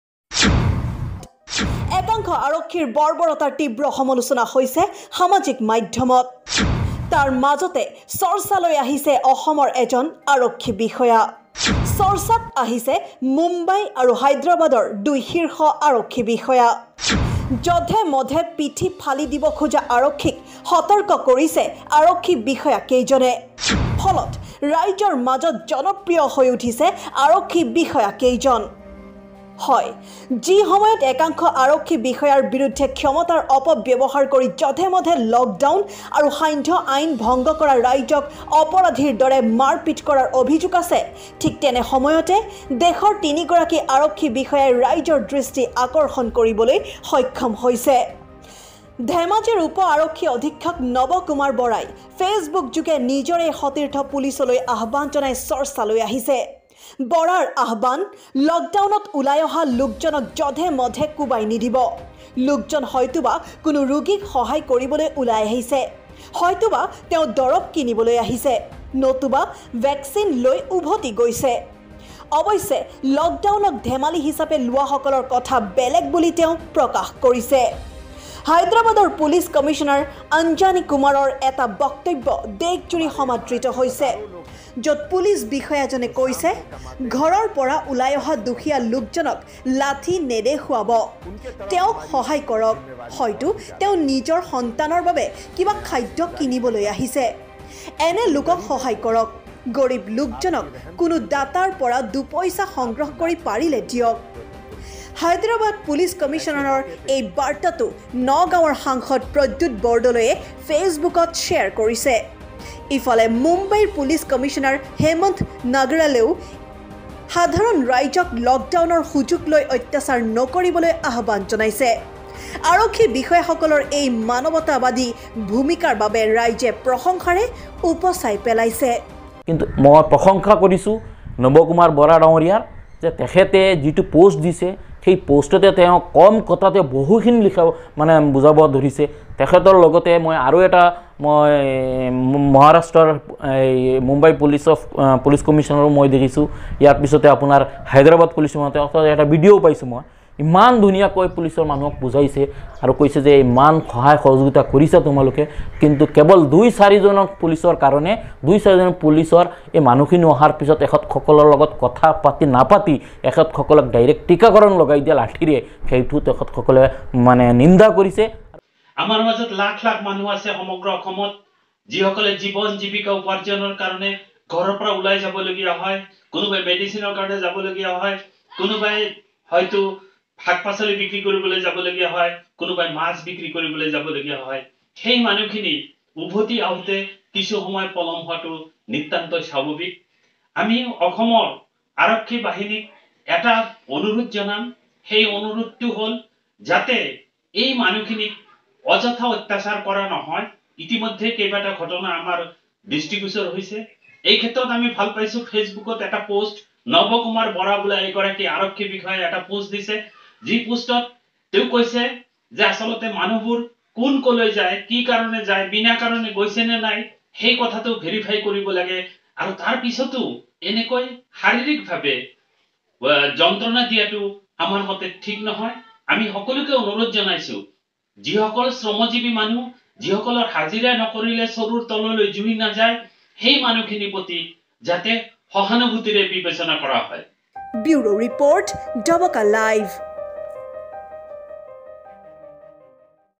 চৰ্চাত তিনি আৰক্ষী বিষয়া ৷
সমগ্ৰ দেশত প্ৰশংসিত হৈছে তিনি আৰক্ষী বিষয়া ৷ কিন্তু কিয় প্ৰশংসাৰ পাত্ৰ হ'ল আৰক্ষী বিষয়া কেইজন? চাওঁ আহক আমাৰ এক বিশেষ প্ৰতিবেদন ৷#assampolice#nabakumarborah#mumbaipolice#haidarabadpolice#